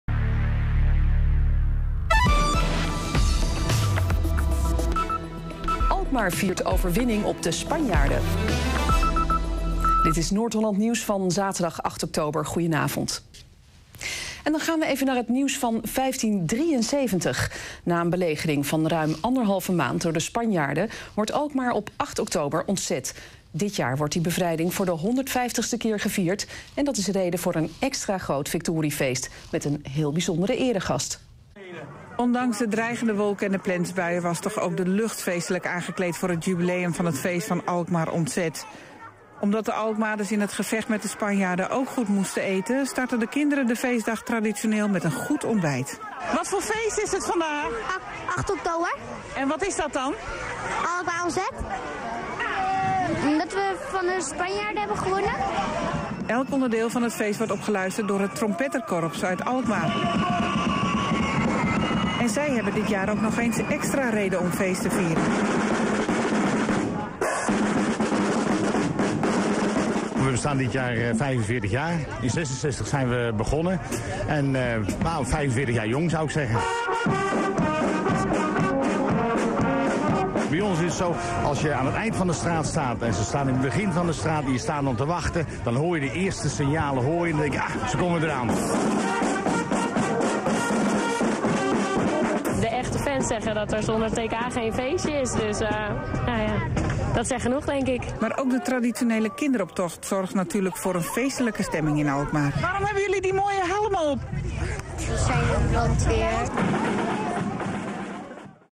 Trompetterkorps Alkmaar Juniorenband bij Alkmaars Ontzet
RTVNH-Nieuws besteed aandacht aan de 8 oktober viering in Alkmaar en aan het "Trompetterkorps Alkmaar"  in het bijzonder.